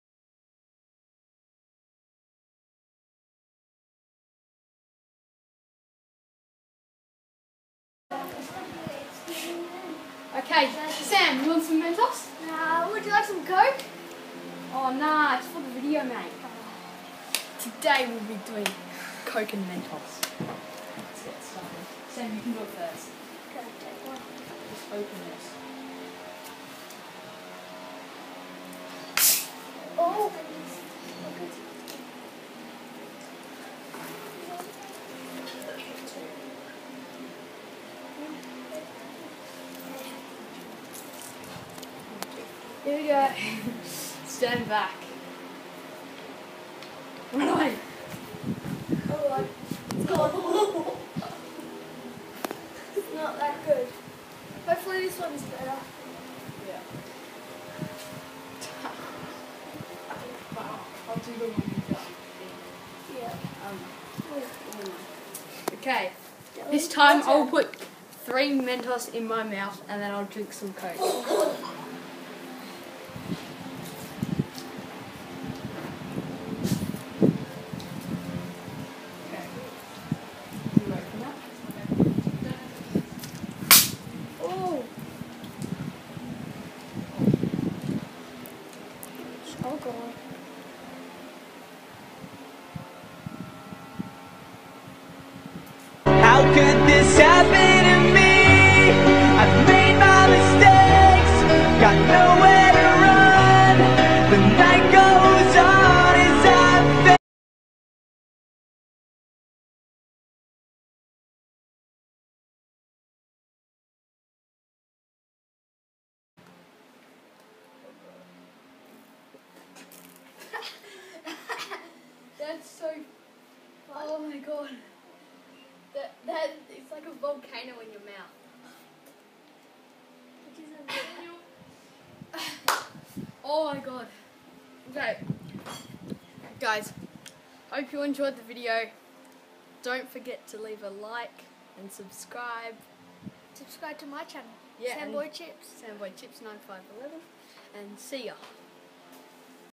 COKE AND MENTOS WITH SAMBOYCHIPS
This is a Coke and Mentos experiment with SAMBOYCHIPS.